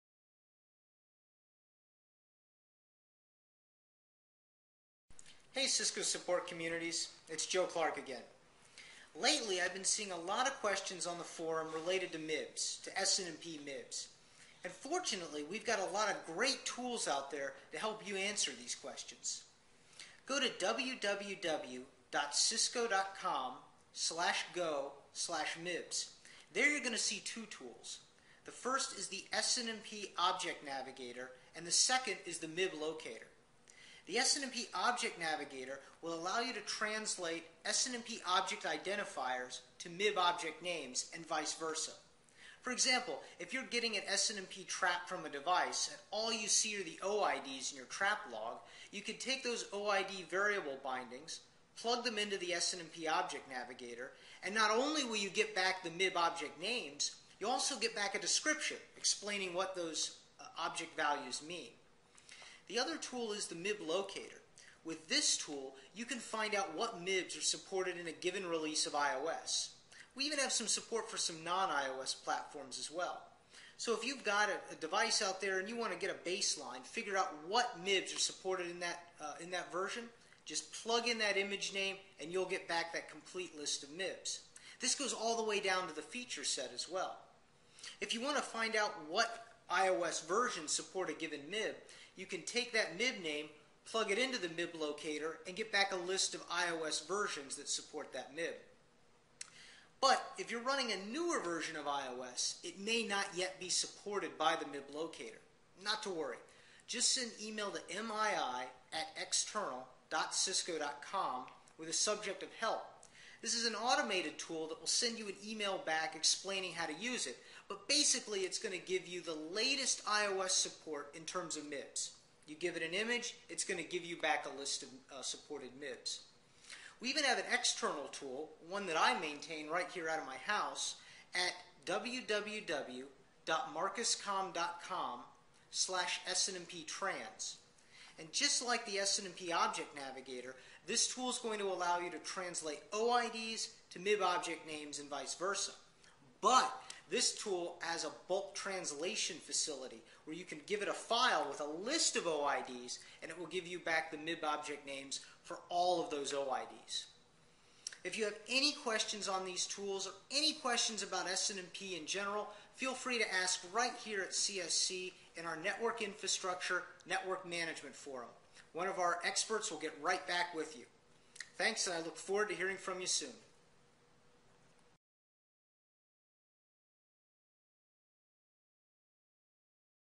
Tips on Finding the Right MIB
Joe Clarke is a Cisco Distinguished Support Engineer. In this video blog, Joe gives quick tips on how to find the right MIBs and explains few tools that can help you like the "SNMP Object Navigator."